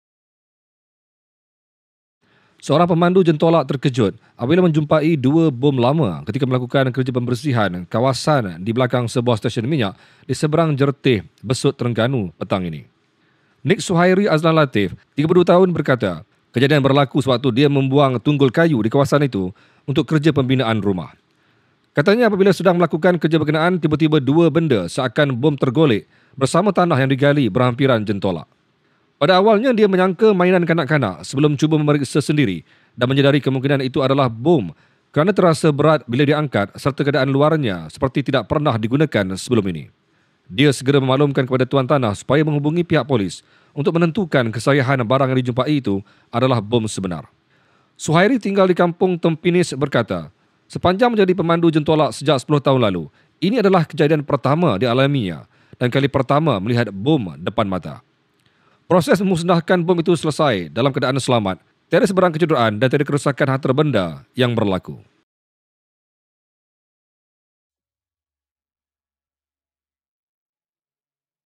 'Tiba-tiba benda seakan bom tergolek'
Seorang pemandu jentolak terkejut apabila menjumpai dua bom lama ketika melakukan kerja pembersihan kawasan di belakang sebuah stesen minyak di Seberang Jertih, petang ini.